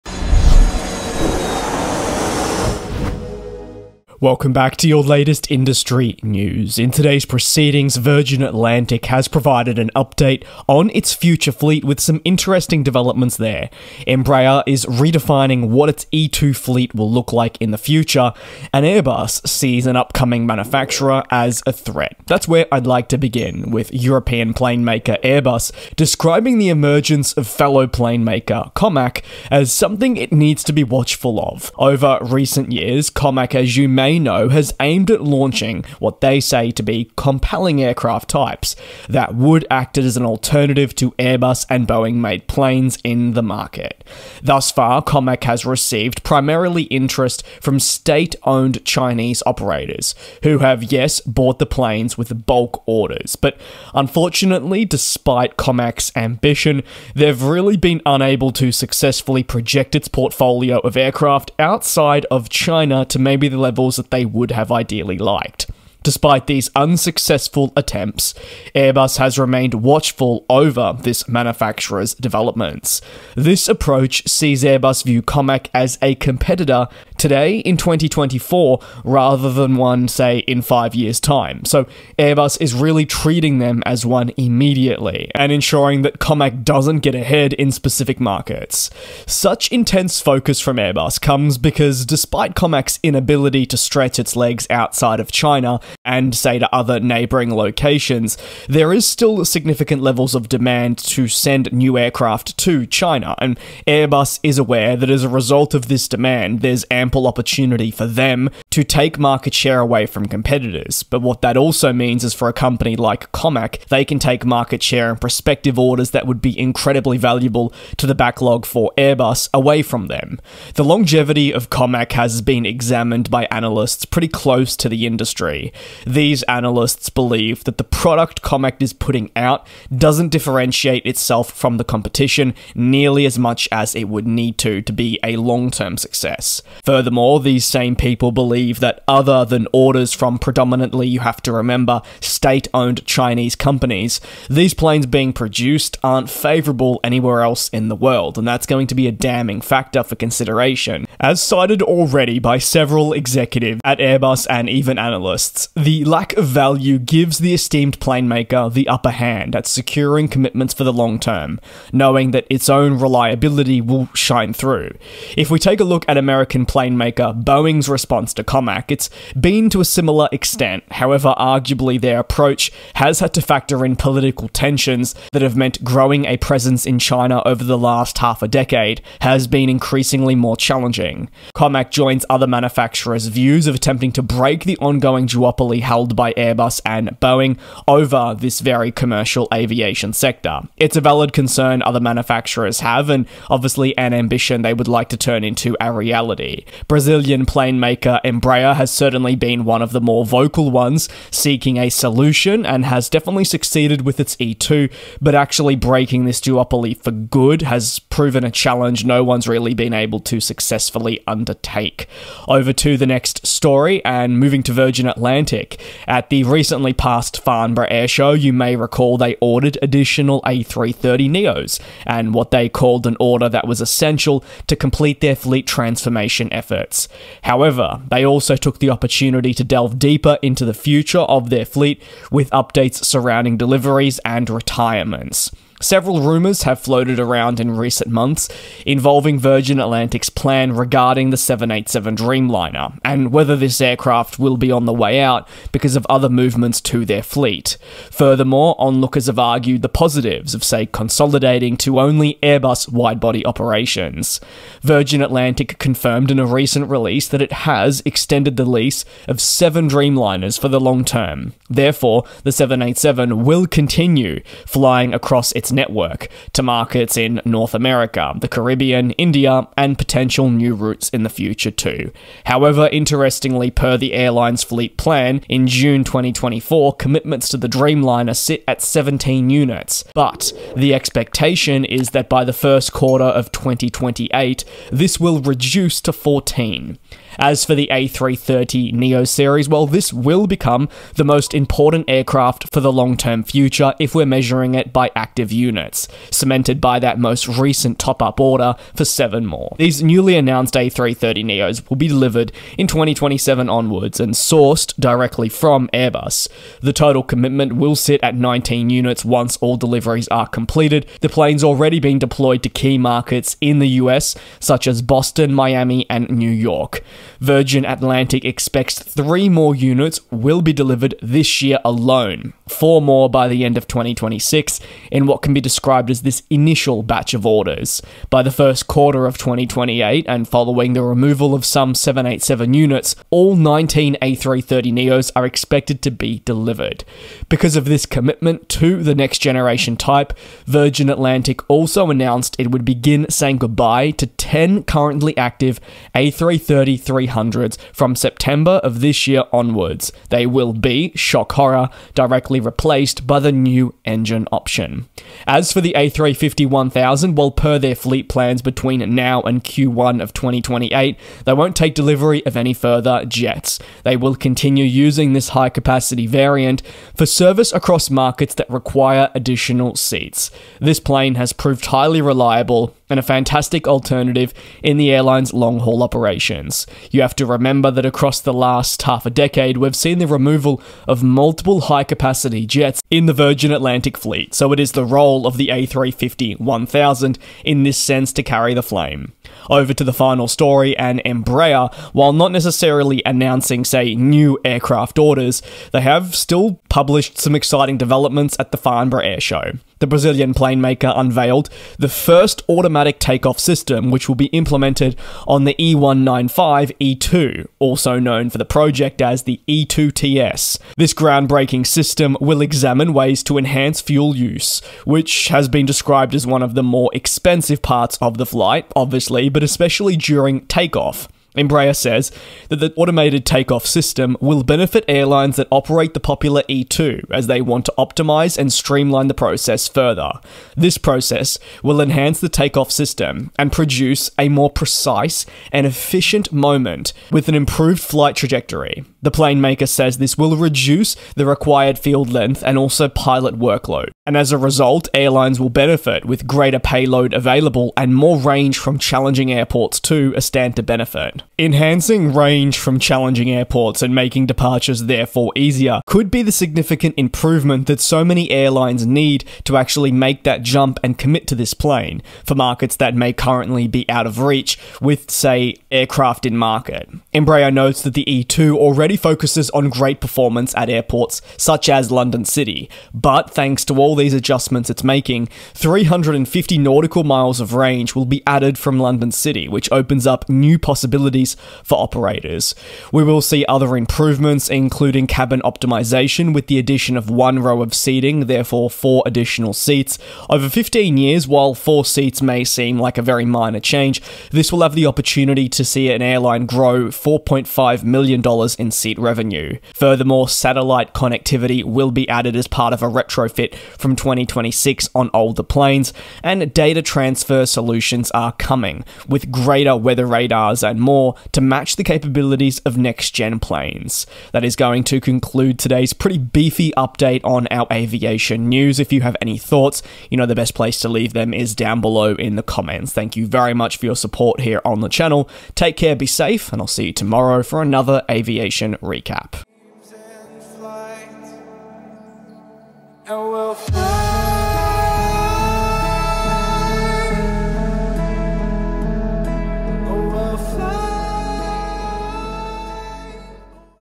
Airbus Rival, Virgin Atlantic Updates & Embraer Automated Takeoffs
Virgin Atlantic has provided an update on its fleet with aircraft set to be retired, new deliveries and further adjustments in the future. Meanwhile, Airbus sees Comac as a threat despite its criticisms of the manufacturer. Airbus is wary of Comac's grip on Chinese and neighbouring markets. Lastly, Embraer will offer automated takeoffs to its pilots aboard E2 aircraft as part of new upgrades being unveiled to improve the series.

A sincere thank you to all the patrons, channel members and the wider community for your support.